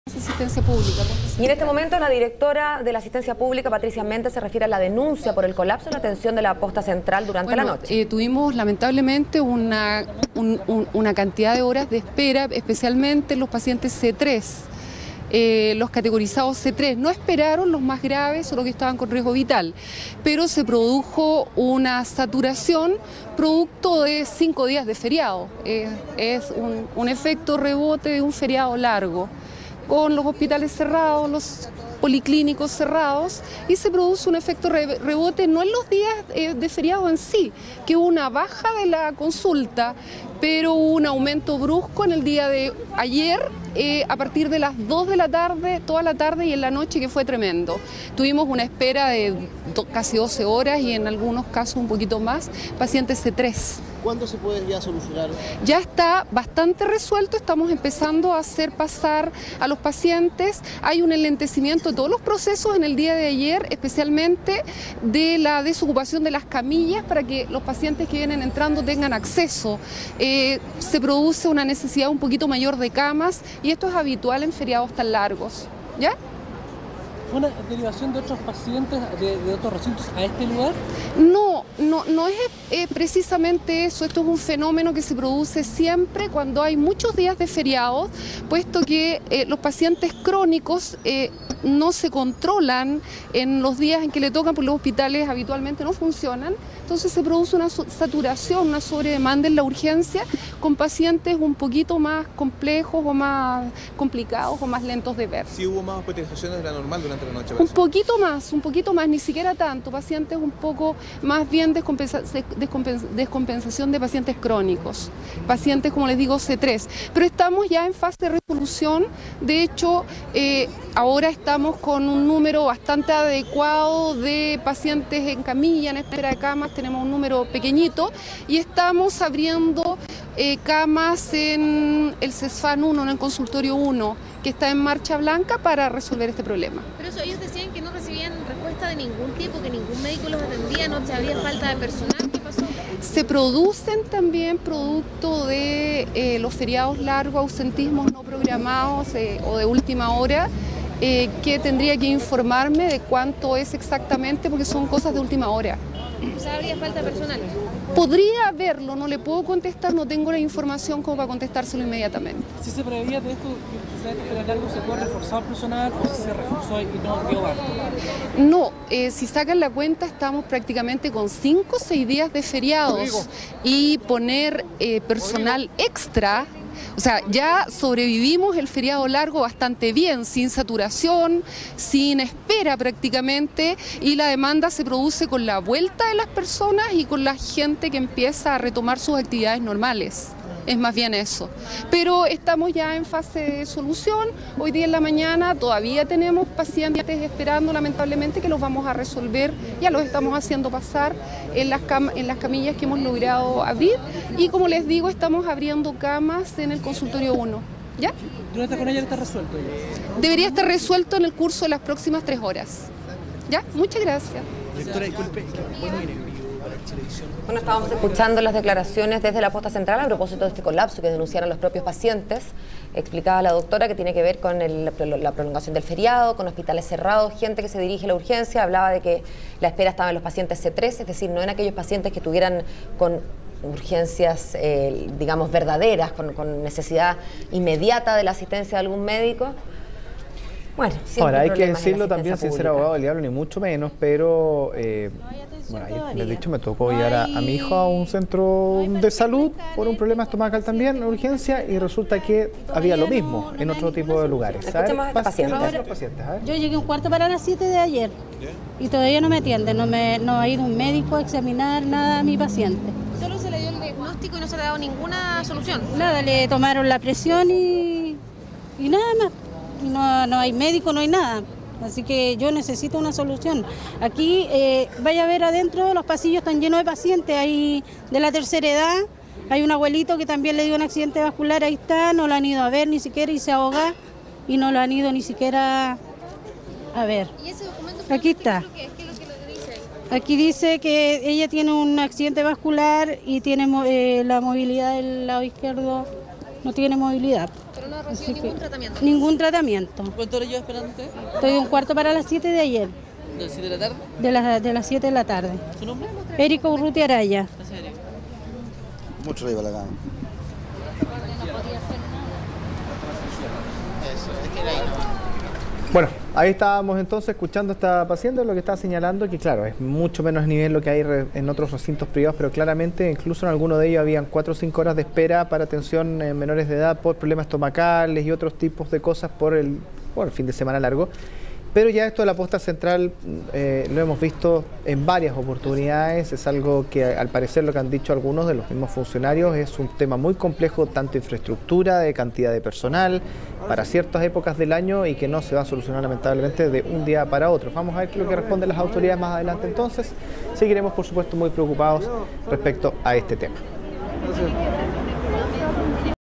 Dir. Asistencia Pública por colapso en ex Posta Central: Hubo "saturación" por Fiestas Patrias
Pacientes denunciaron esperas de hasta 14 horas en el recinto de salud.